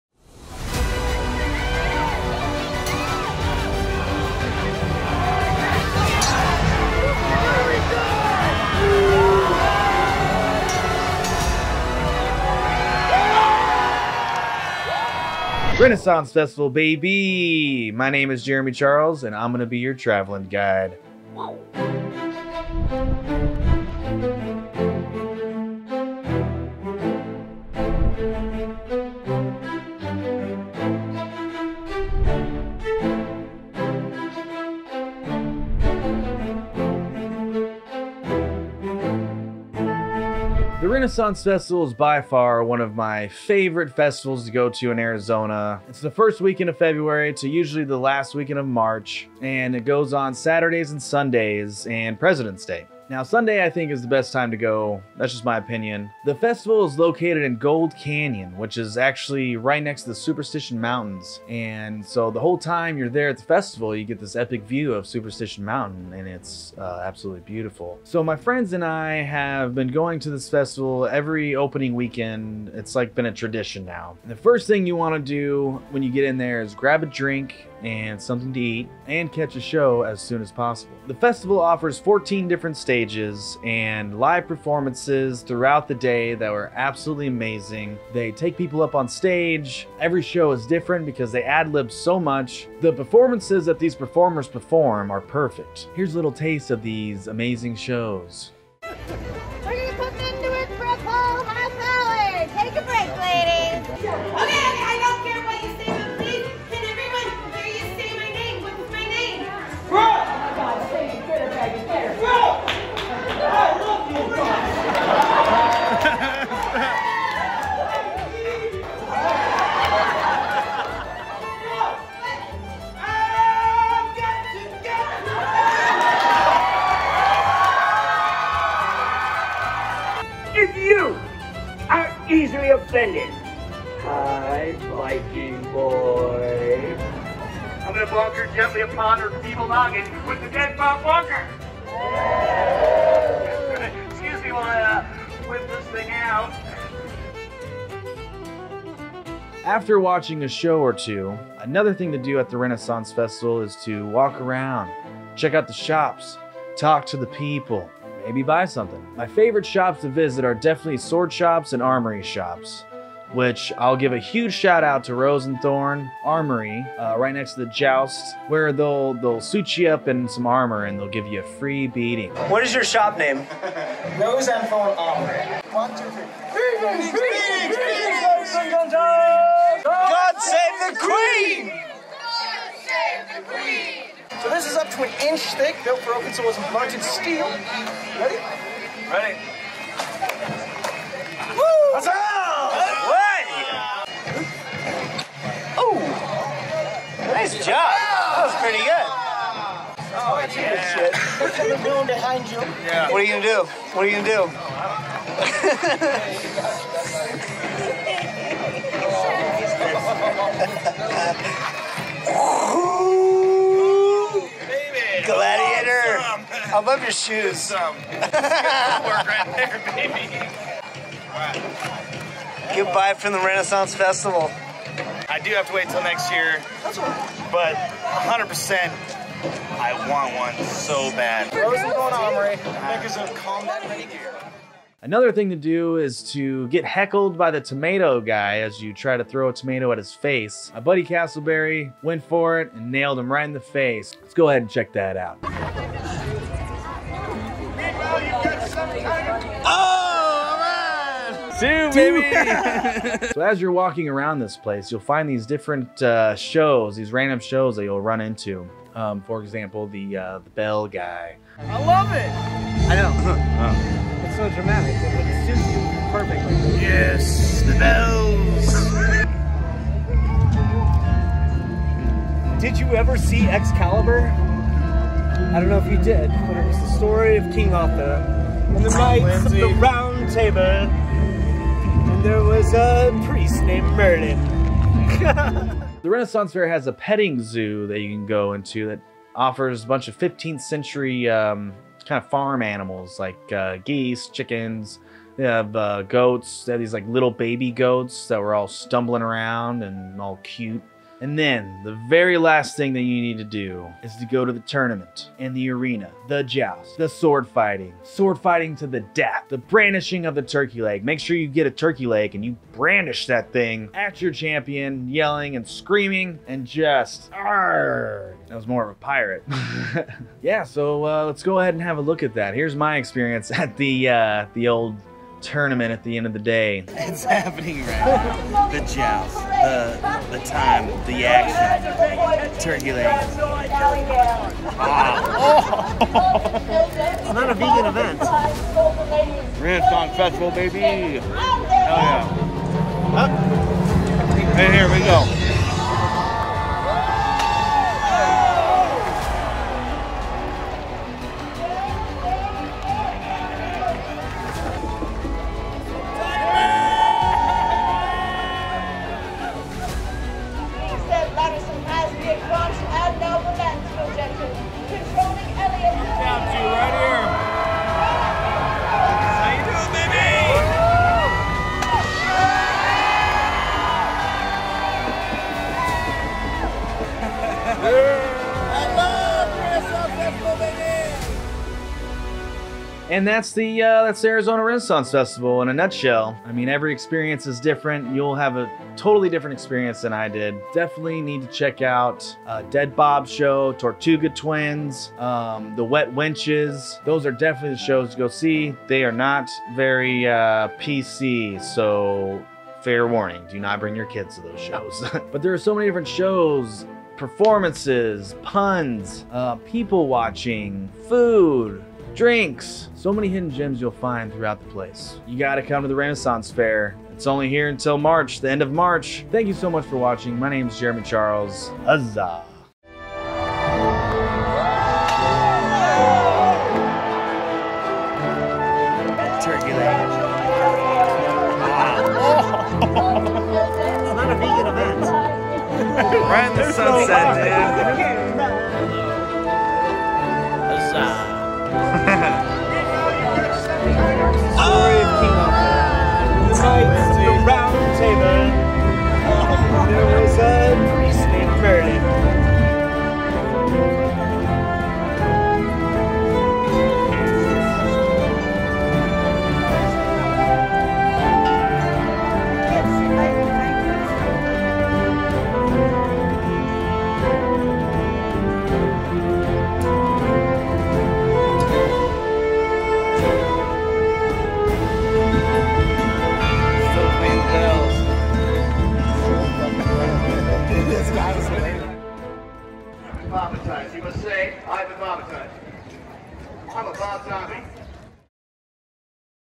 AZ Renaissance Festival
Arizona Renaissance Festival! Turkey legs, stage performances, beer, mead, the tournament! What more could you ask for!? This is a rundown of what you can expect at the Arizona Renaissance Festival! Huzzah!